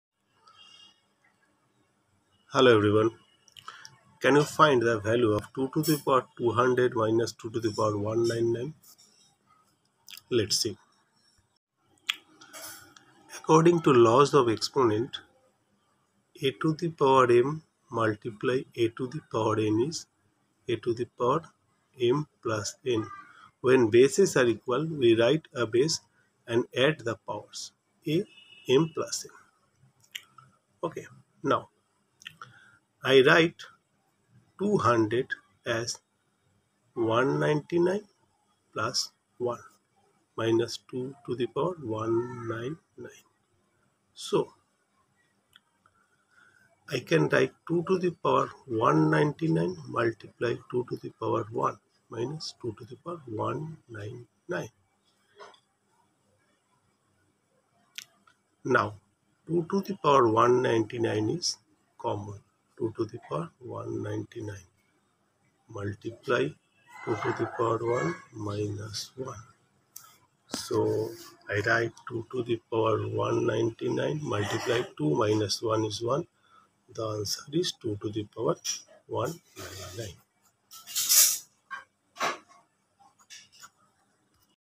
2²⁰⁰ - 2¹⁹⁹ =? | Sums On Exponent | Junior Maths Olympiad | Harder Sums On Indices
2²⁰⁰ - 2¹⁹⁹ =? | Sums On Exponent | Junior Maths Olympiad | Harder Sums On Indices | Sums On Indices